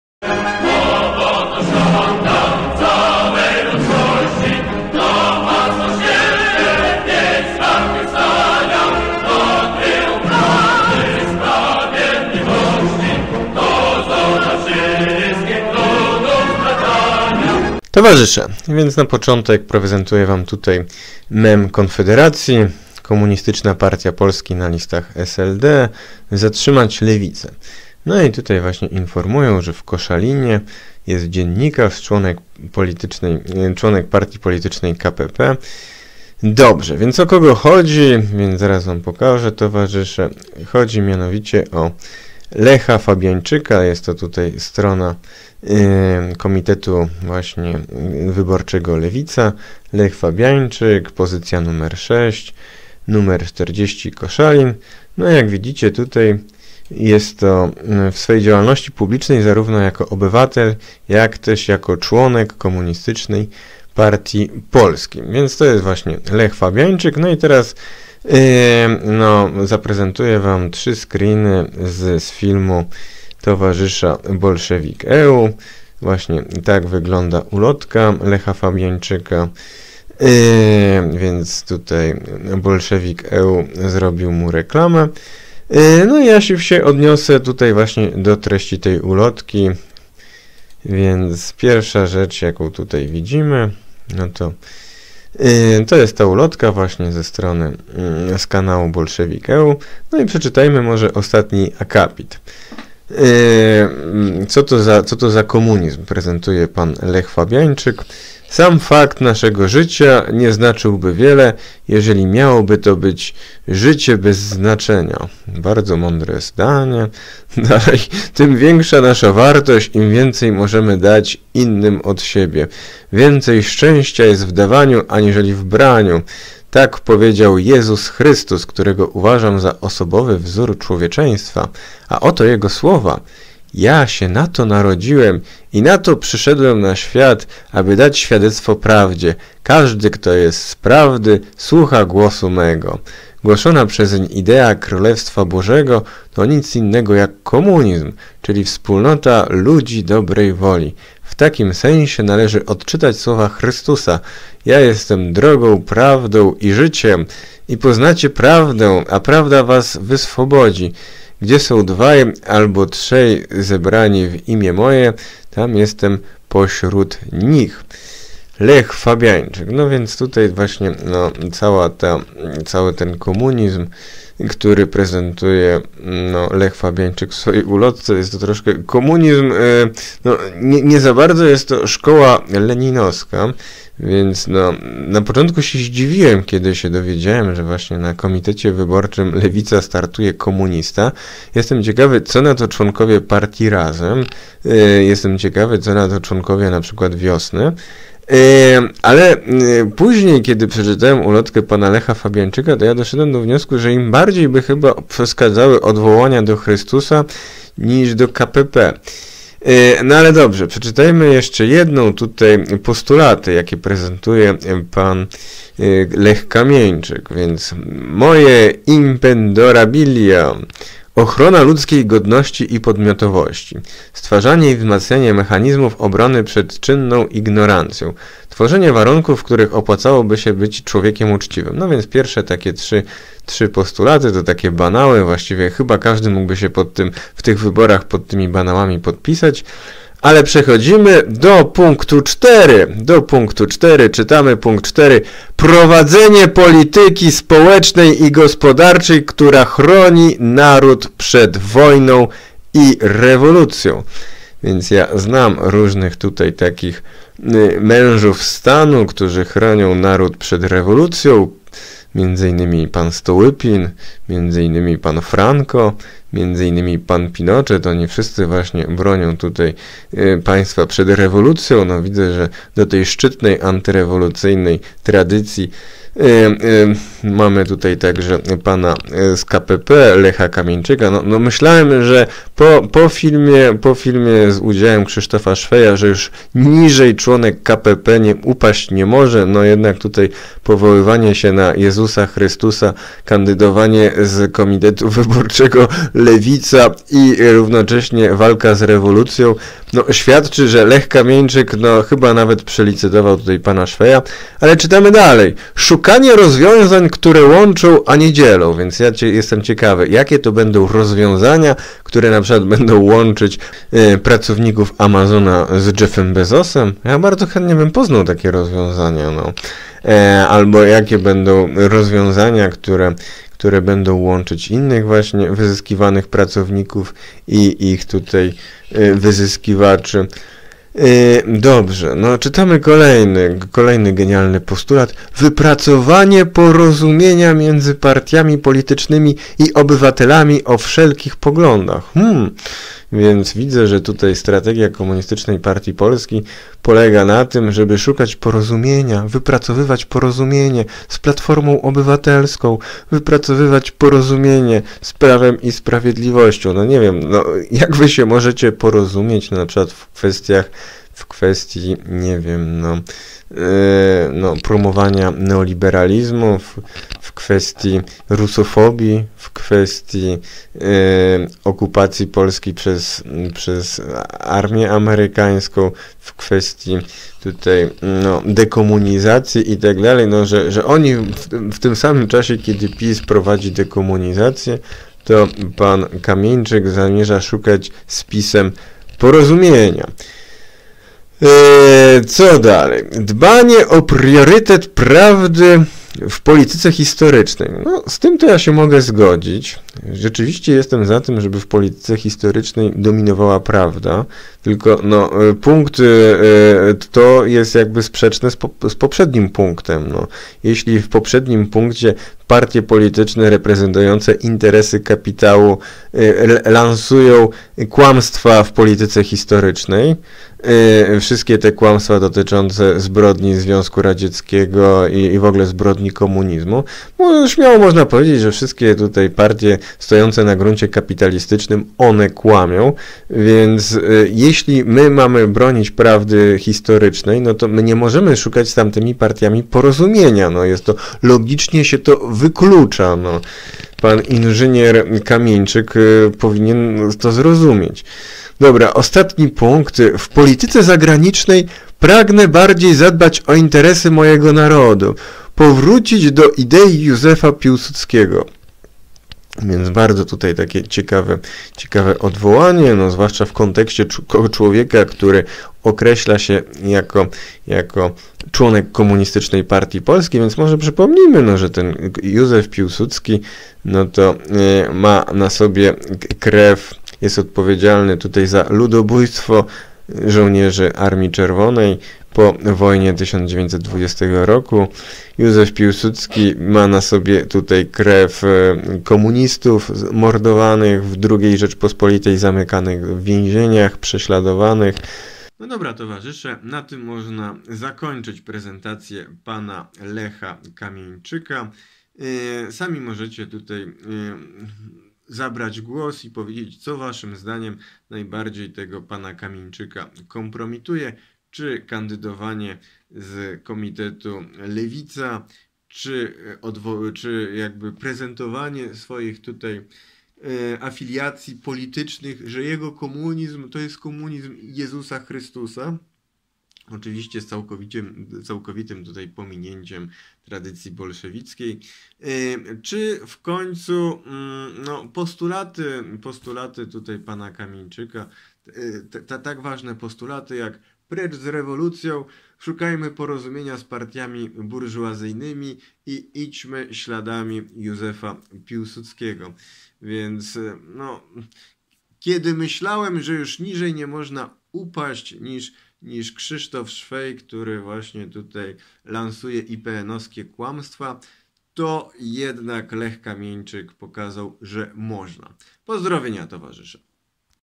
Komunista na listach SLD w Koszalinie
Lech Fabianczyk, czlonek KPP, kandyduje z Koszalina z list Lewicy.